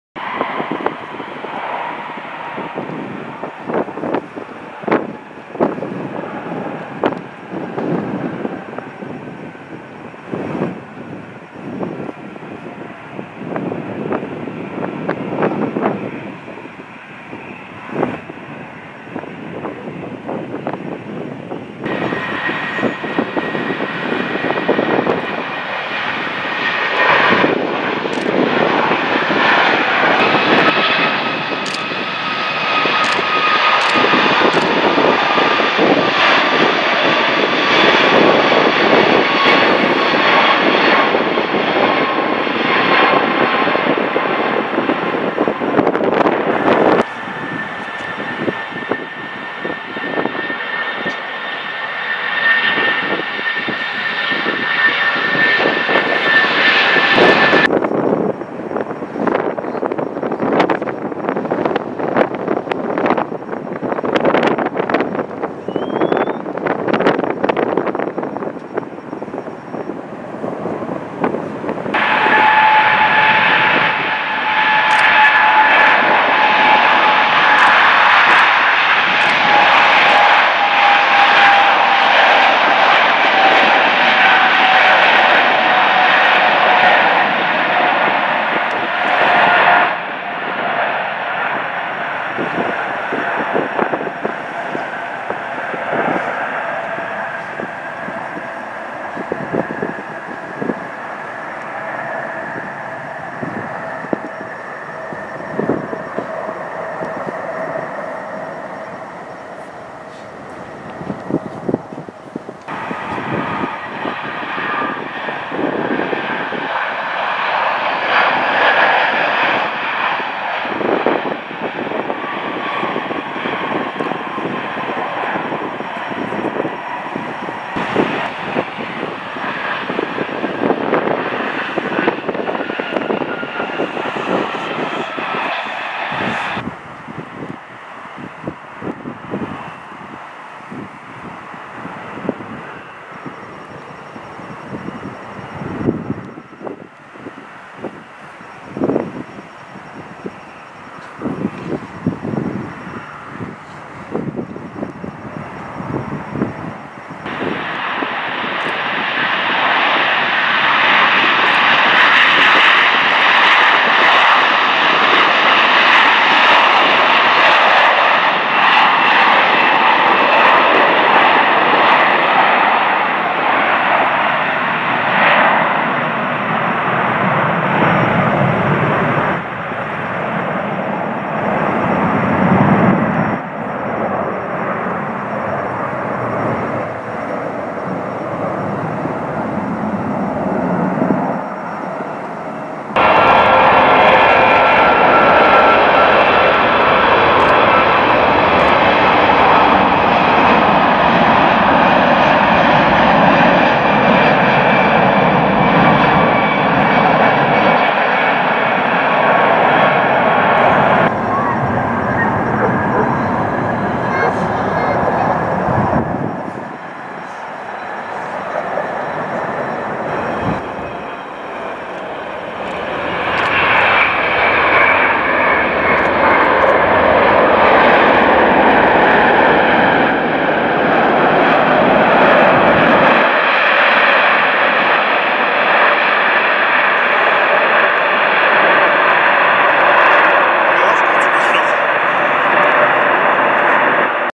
Planespotting at Munich airport (incl. Air France Boeing 777-300ER)
Some planespotting-impressions of Munich International airport (MUC/EDDM) including Meridiana Boeing 737-700,  Etihad Airways Airbus A340-600, Thai Airways Boeing 747-400, Singapore Airlines Boeing 777-300ER, Air China Airbus A330-300, Qatar Airways Airbus A350-900,...
*The Air France Boeing 777-300ER was enroute from Paris to Bangkok as AFR166 and diverted to Munich due to medical reasons.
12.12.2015